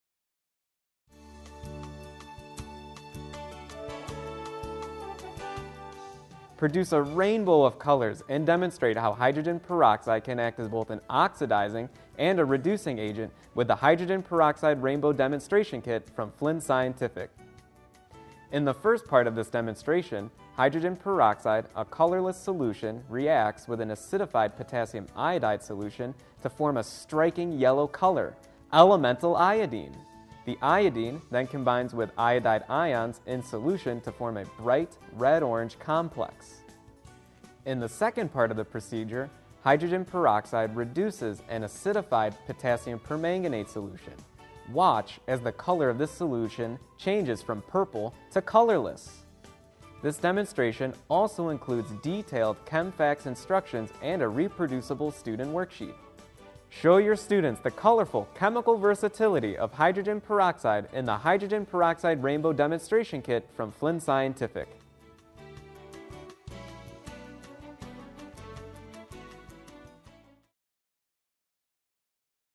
Hydrogen Peroxide Rainbow
Watch this demonstration of the hydrogen peroxide rainbow.